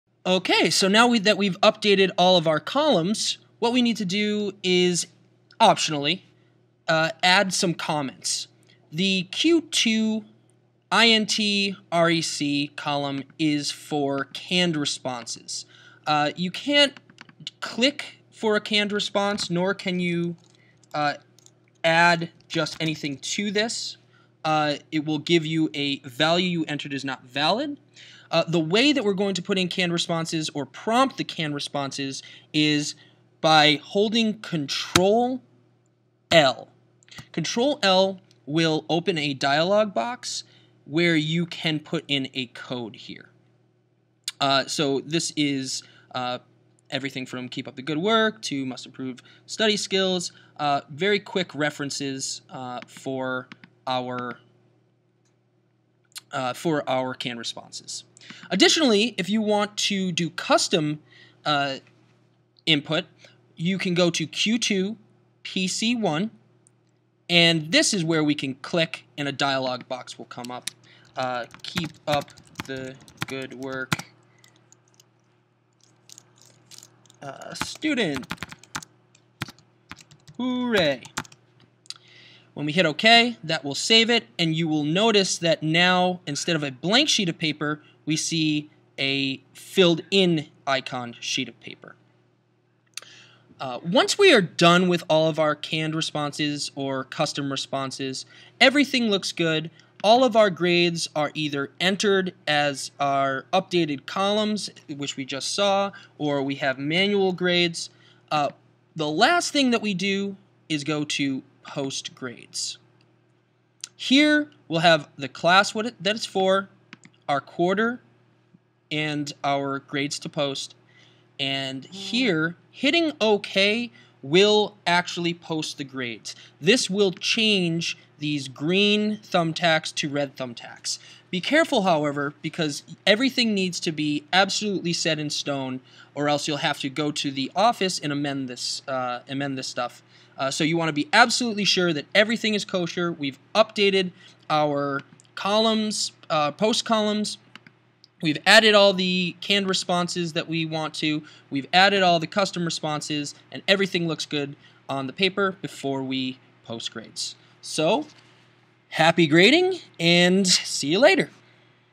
Aspen Tutorials - Progress Reports: Adding Comments and Posting Grades
In this tutorial, we learn how to add canned and custom comments to our progress reports, then post our finalized grades.